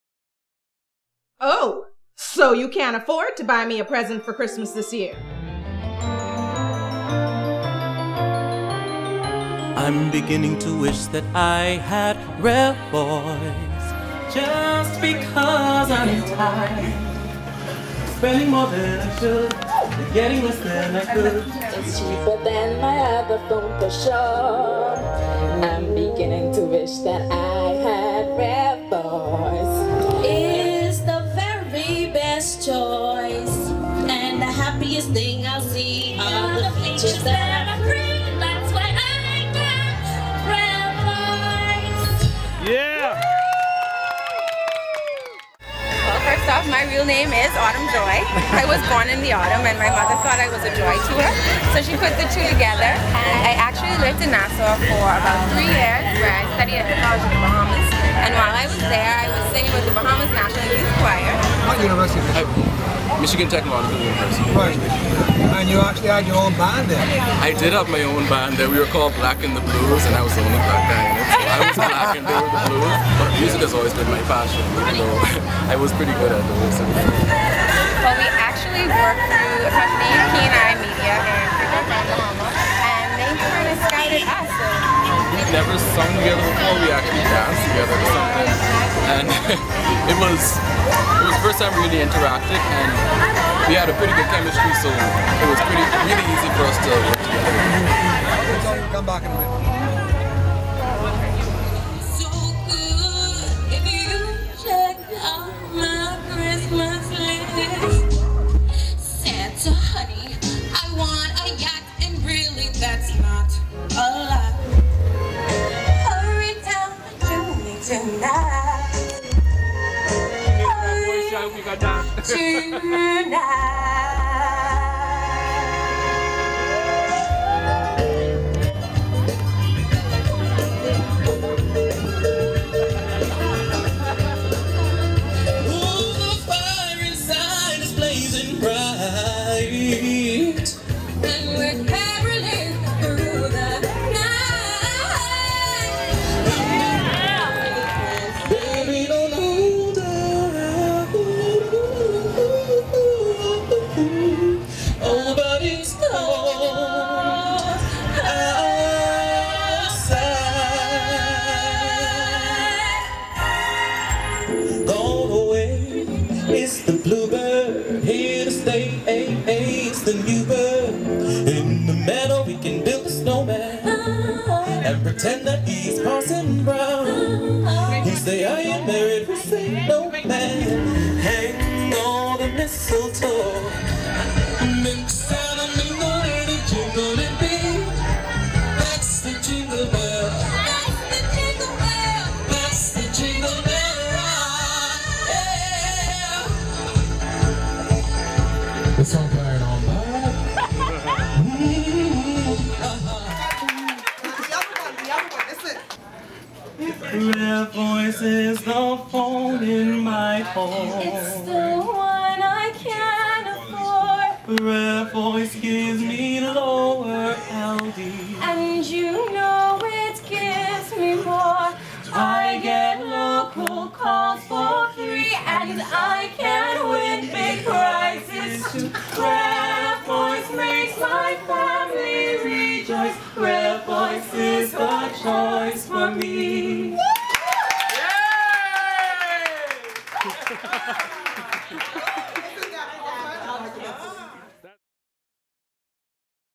REJOICE with REVoice - LIVE in Nassau (Dec 8, 2012)
We had a blast in Nassau on Saturday Dec 8! It was our first REJOICE with REVOICE - LIVE! With our live broadcast on Hot 91.7FM and on Cable 12, we had a great time -- kids, parents, friends and family, all celebrating the Christmas season with REVOICE. Greg Lampkin was our host on radio and TV -- great job, Greg!

Tony Lowe and Autumn-Joy Dames were on hand to sing and entertain the crowds with their great selection of Christmas favorites. It was truly a joy to hear them!

And of course there was our singing competition -- we had a blast and gave away an Apple iPad and iPod Touch!

At the end of the day, Tony & Autumn-Joy wowed the Cable Bahamas staff in Customer Care...watch for that at the end. :)

Here's a little video with clips from the day -- hope you have as much fun watching as we did doing it!